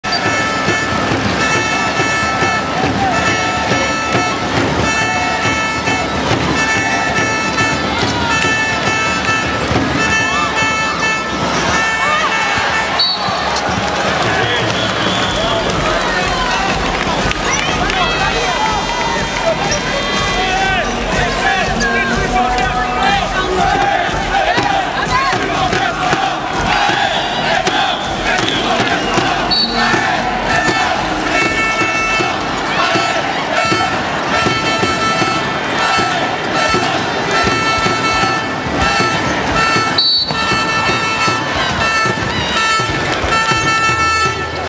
Vidéo 360 Flammes Bercy02
Vidéo 360° Les Flammes Carolo battue par Bourges en finale de la coupe de France à Paris Bercy